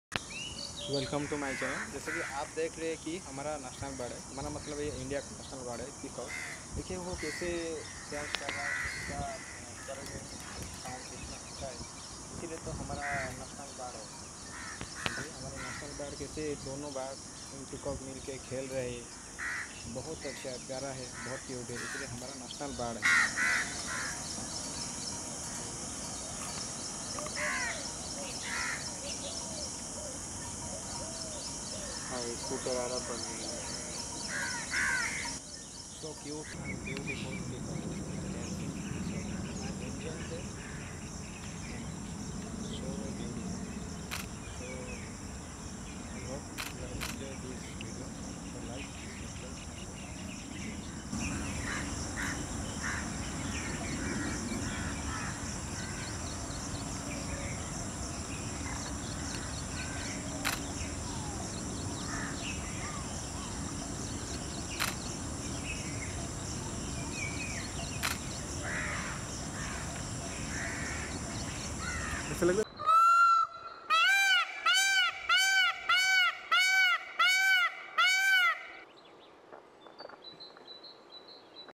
beautiful# national bird# peacock#squalling# cute birds#shorts#
the beautiful Indian national birds 🐦 are dancing and singing moments .
the green peacock are singing and two peacock are dancing with each other.finally we are know that the peacock are dancing the moments of rainfall.